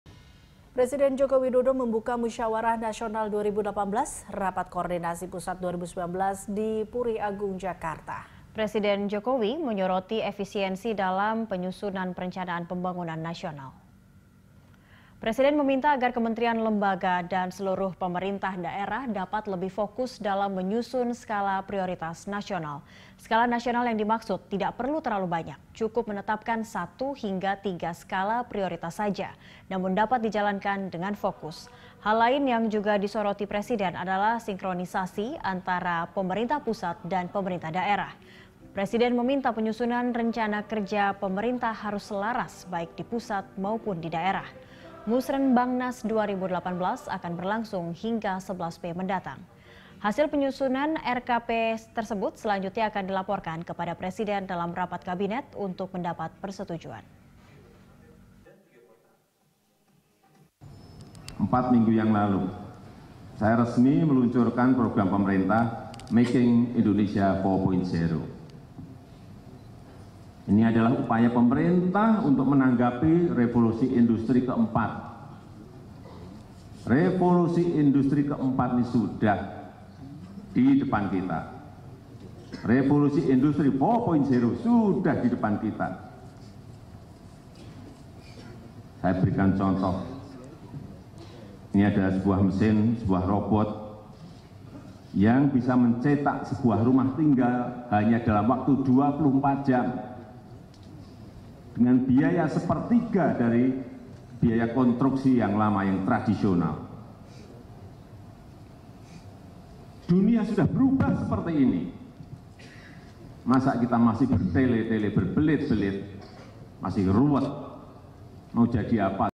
Buka Musrenbang, Jokowi Soroti Sinkronisasi Pusat dan Daerah
Presiden Joko Widodo membuka Musyawarah Nasional 2018, Rapat Koordinasi Pusat 2019 di Puri Agung, Jakarta. Presiden Jokowi menyoroti efisiensi dalam penyusunan perencanaan pembangunan nasional.

Facebook.com/BeritaSatuTV
Youtube.com/BeritaSatu